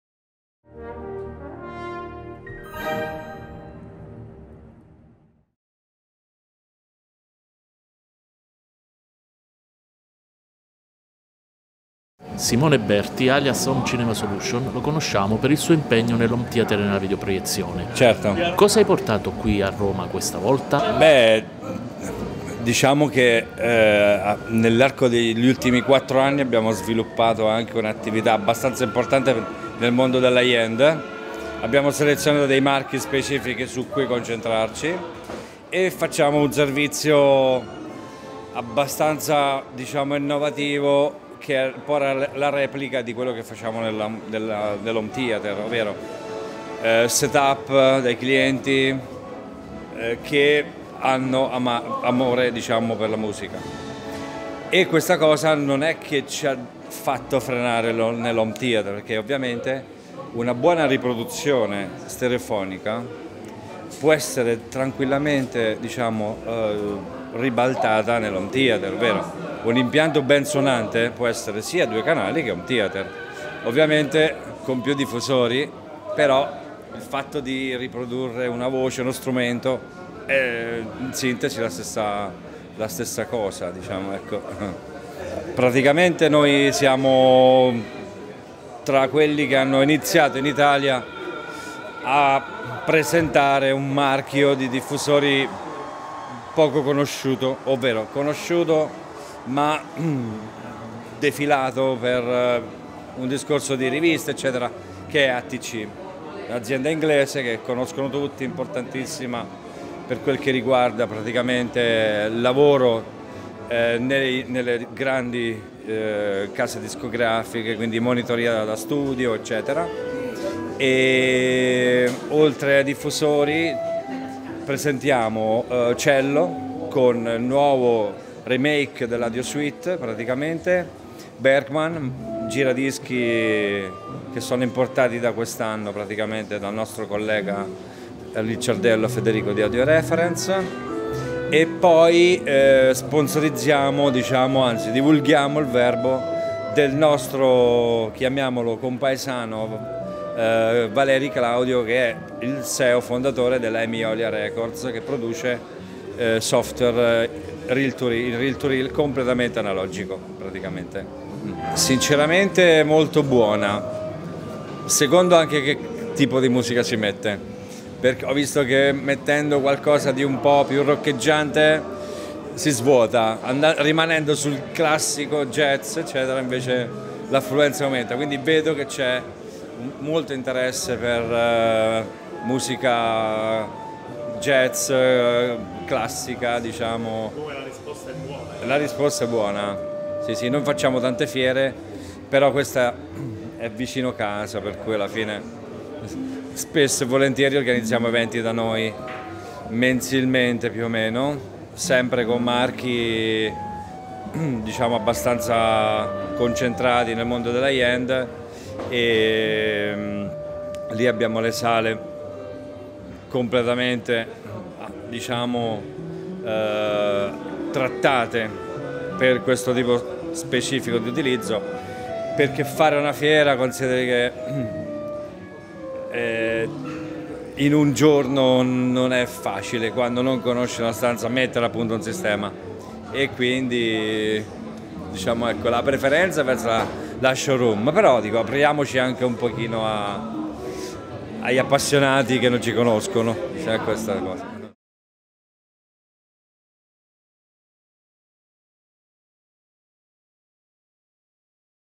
Roma Hi-fidelity 2019: Simone Berti - Homecinemasolution
Simone Berti volto noto nel mondo dell'Homevideo, presenta a Roma durante la manifestazione sull'alta fedeltà un impianto esclusivamente audio con giradischi Bergmann Galder T.T. , DAC/lettore di rete dCS Bartók, server Innuos Zen MKIII, pre ATC SCA2, speaker ATC SCM 100 ASL e finale a 6 canali ATC P6